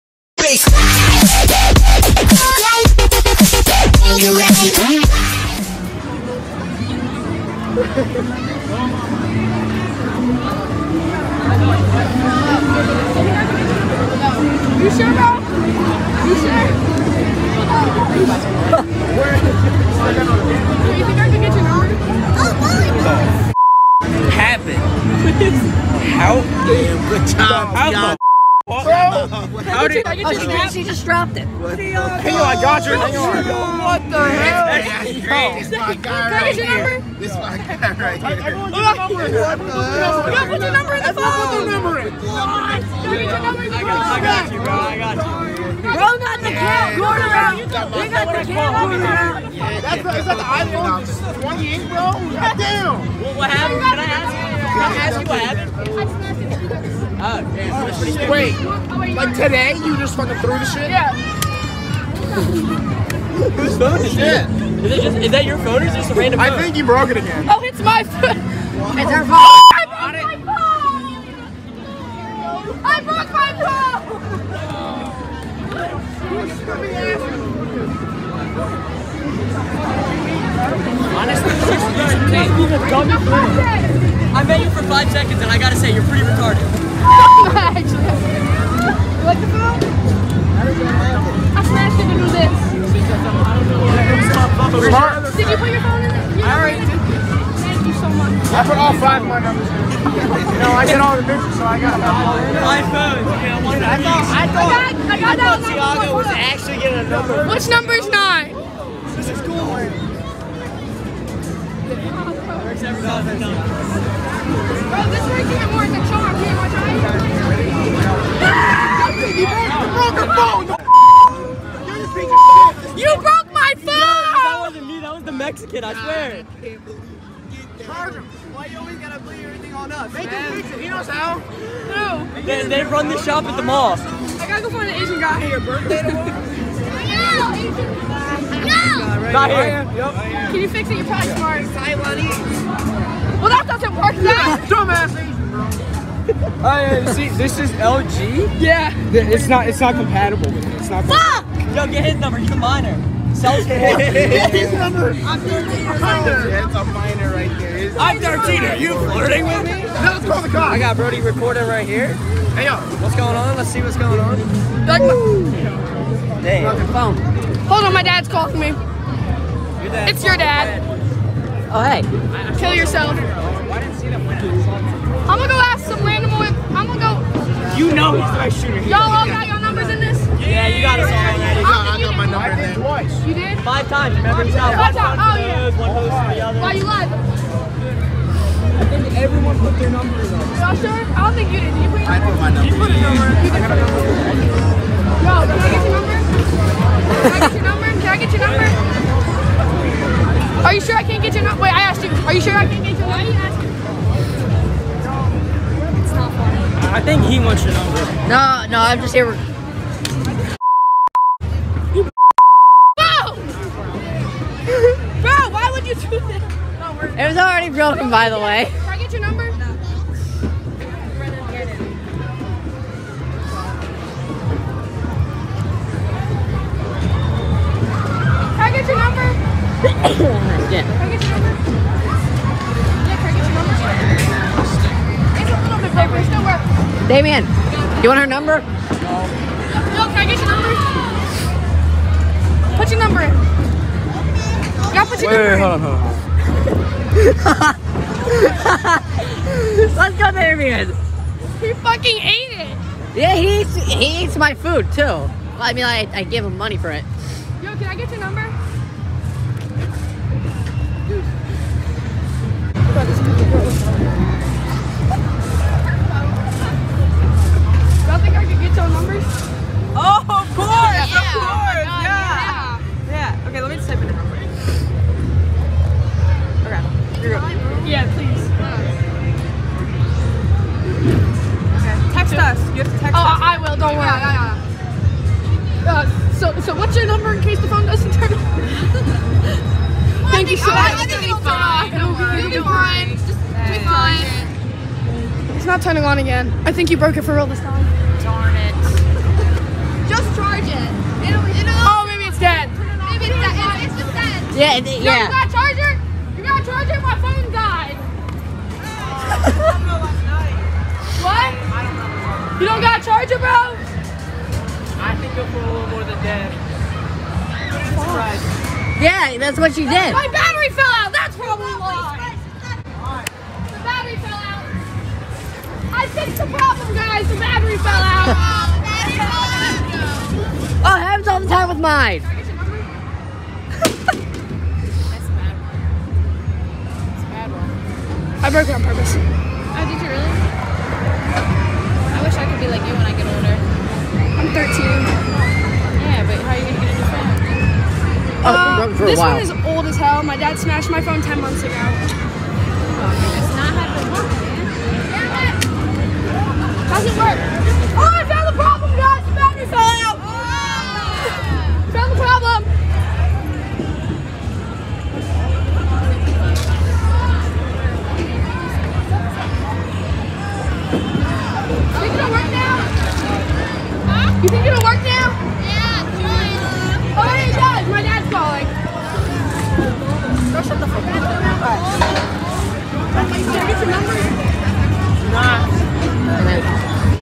My friend did a broken phone prank
This is funny, my friends phone broke a while back and she decided to play a prank at a carnival. Something for people to have a laugh at.